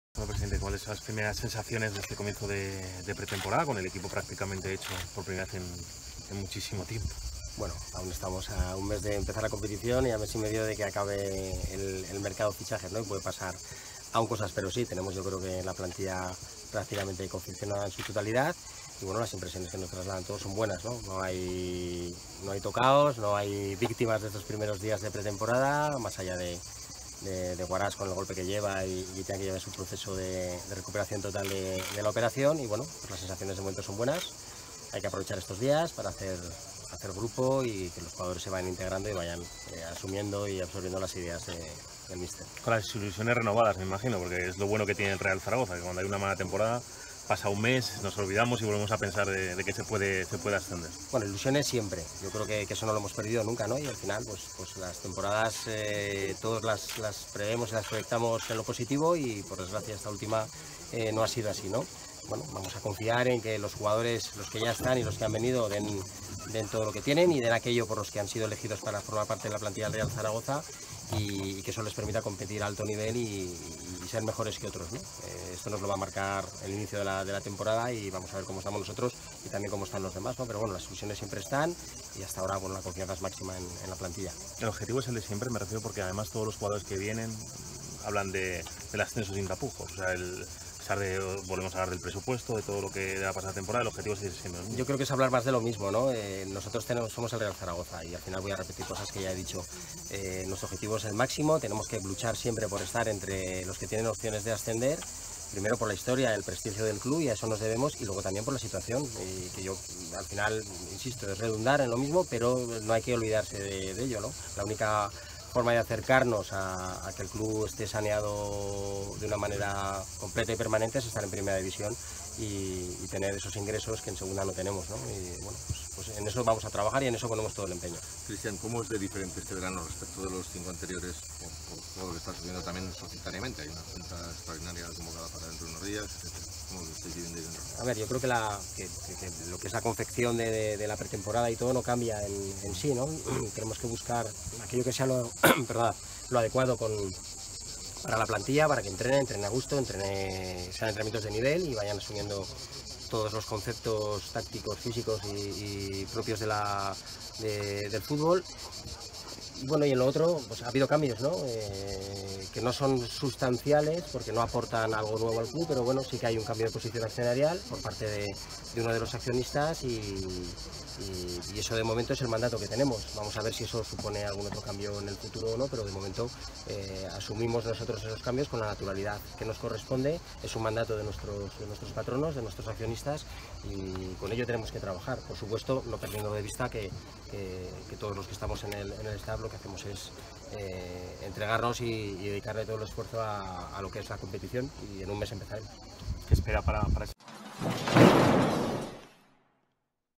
Lapetra Analiza la Pretemporada y la Plantilla del Real Zaragoza
El presidente del Real Zaragoza ha analizado la plantilla y la pretemporada de su equipo.